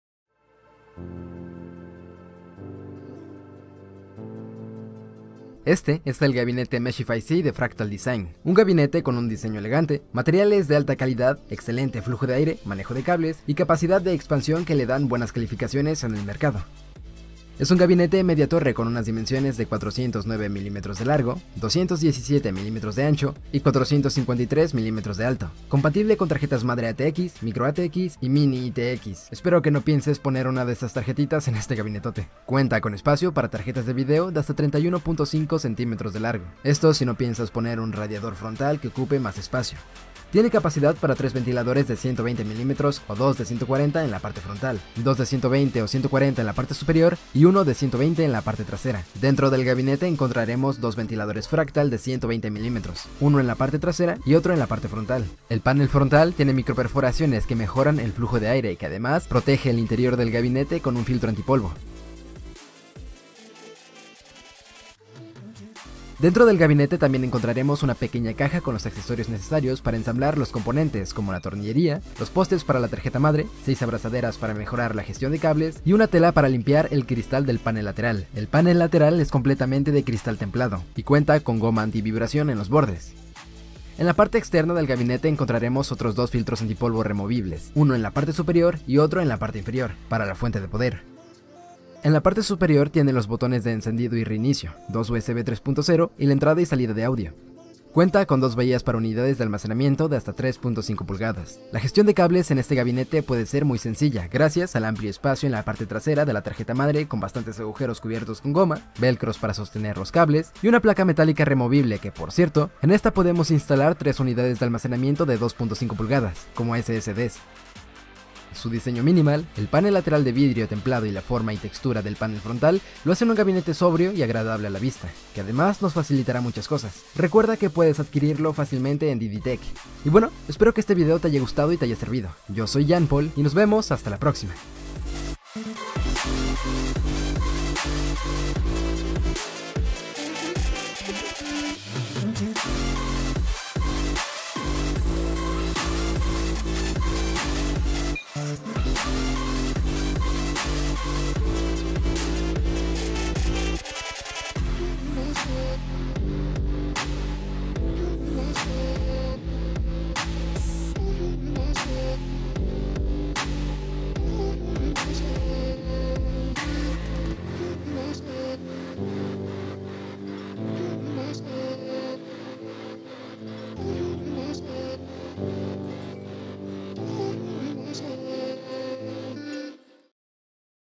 Fractal Design - Meshify C - Un gabinete con verdadero flujo de aire.
Te presentamos el videoreview del gabinete Fractal Design Meshify C